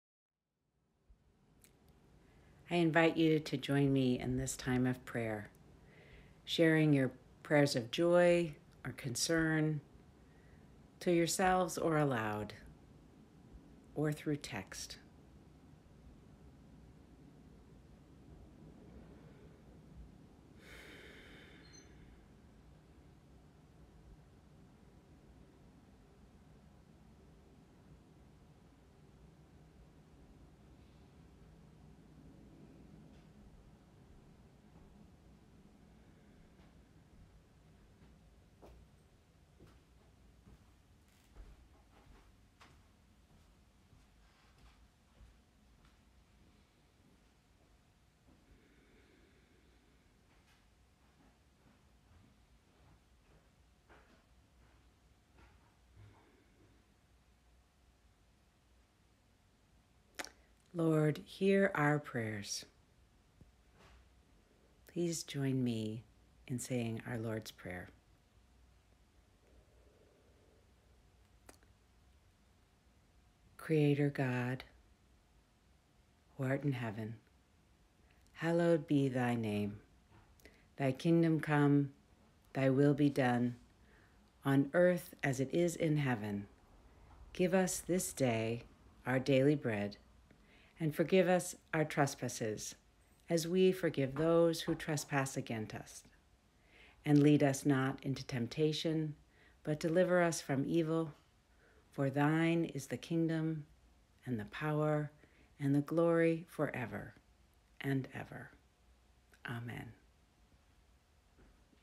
Prayers of the People
Prayers of the People and the Lord's Prayer led by elder Lawrie Williams